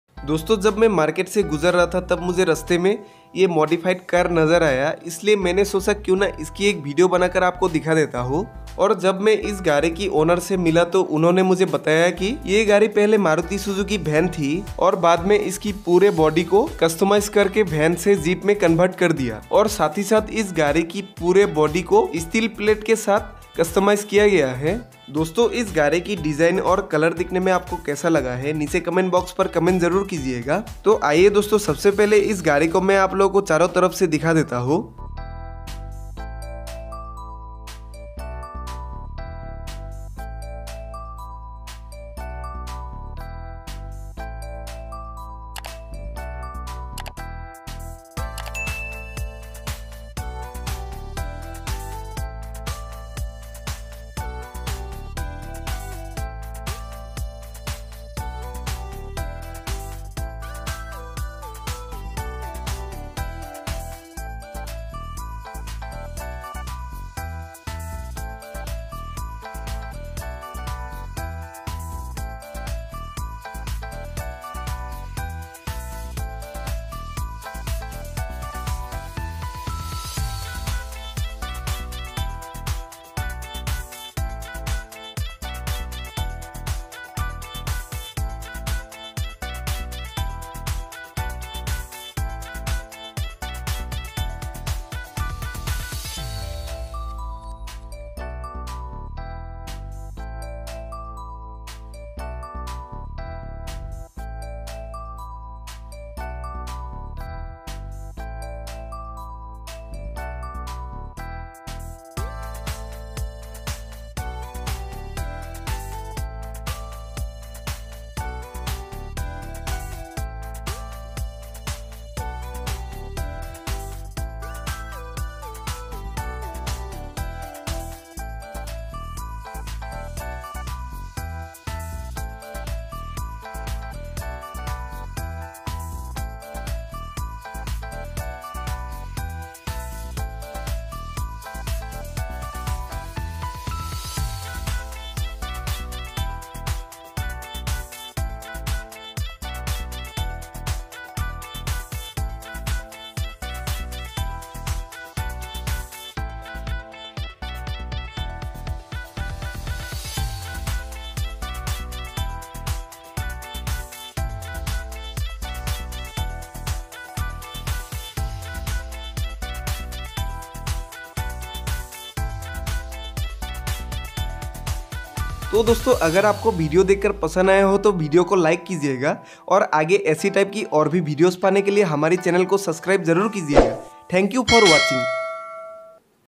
Modified Maruti Van Into Small Jeep | 2023 Full details & Review 😱🔥
Modified Maruti Van Into Small Jeep In 2023 Full details & Review